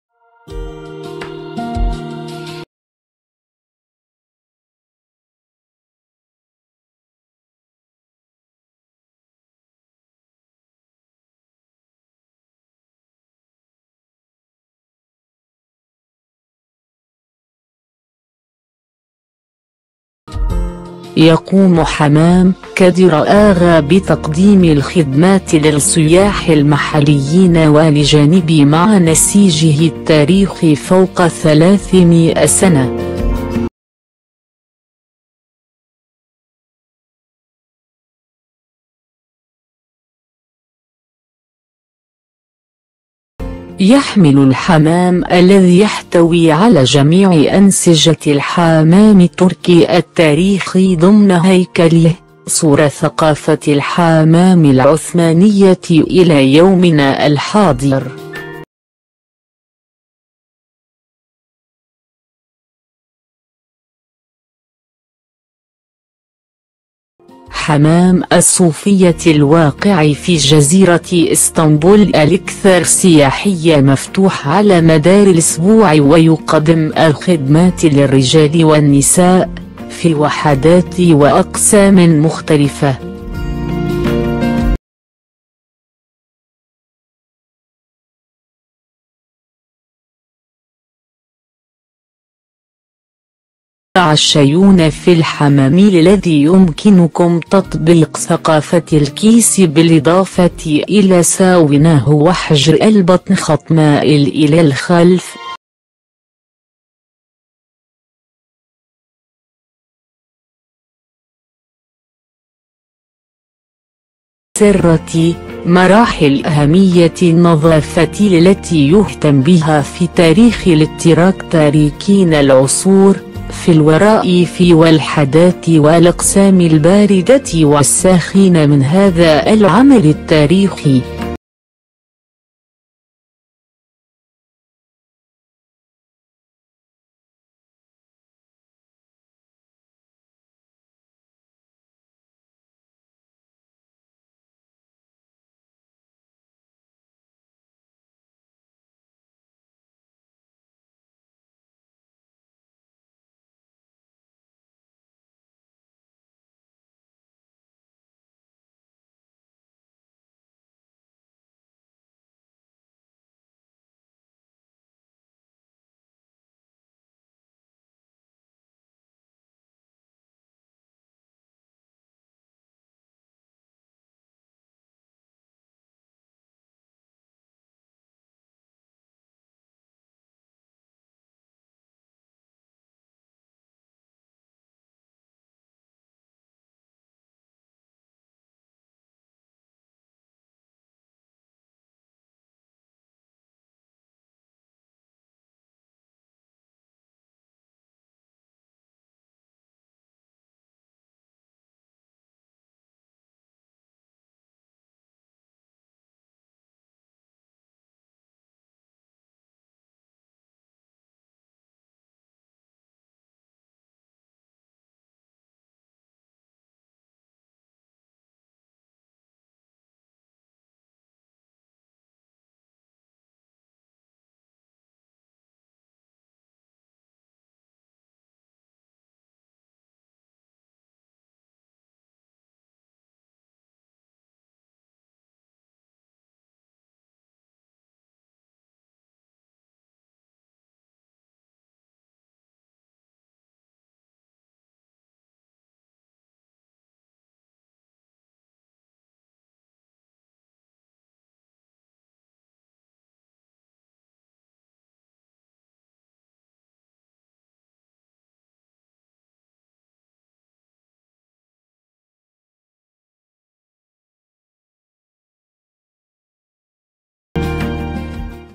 Kadırga Hamamı ²⁰¹³ | الحمامات | حمام تاريخ | حمام إسطنبول
Kadırga Hamamı - الحمامات - حمام تاريخ - حمام إسطنبول

يقوم حمام كادرآغا بتقديم الخدمات للسُياح المحليين والاجانب مع نسيجه التاريخ فوق ثلاثمأة سنة.

. يحمل الحمام الذي يحتوي على جميع أنسجة الحمام التركي التاريخي ضمن هيكله، صورة ثقافة الحمام العثمانية الى يومنا الحاضر.

 حمام الصوفية الواقع في جزيرة إسطنبول الاكثر سياحية مفتوح على مدار الاسبوع ويقدِّم الخدمات للرجال والنساء في وحدات وأقسامٍ مختلفة.

 ستعشيون في الحمام الذي يمكنكم تطبيق ثقافة الكيس بالاضافة الى ساوناه وحجر البطن/

السرة، مراحل اهمية النظافة التي يُهتم بها في تاريخ الاتراك تاركين العصور في الوراء في والحدات والاقسام الباردة والساخنة من هذا العمل التاريخي.

 سيفتخر حمام كادرآغا التاريخي من تضييفكم ضيوفنا الاعزاء الاكارم.